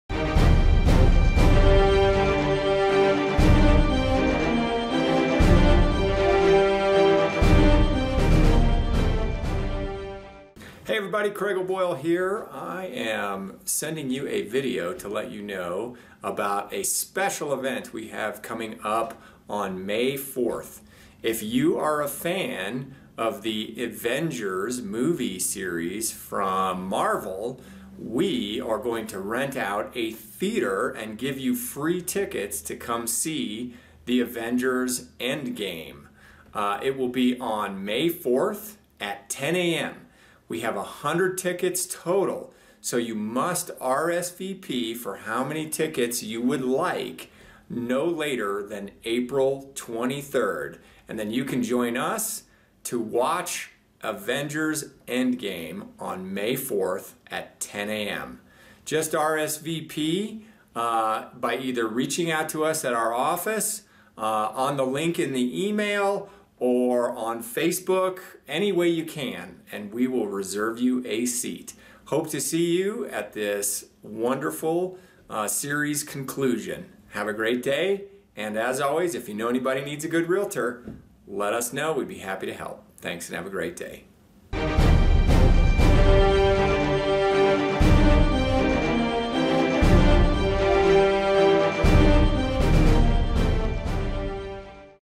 O'Boyle Real Estate Group Client Movie Party
If you are a client of the O'Boyle Real Estate Group, we have free tickets for you to come see Avengers Endgame on May 4th at 10 am.  Call us to reserve your ticket as we only have 100 available.  Oh by the way, if you know someone who needs a good Realtor, don't hesitate to give us a call.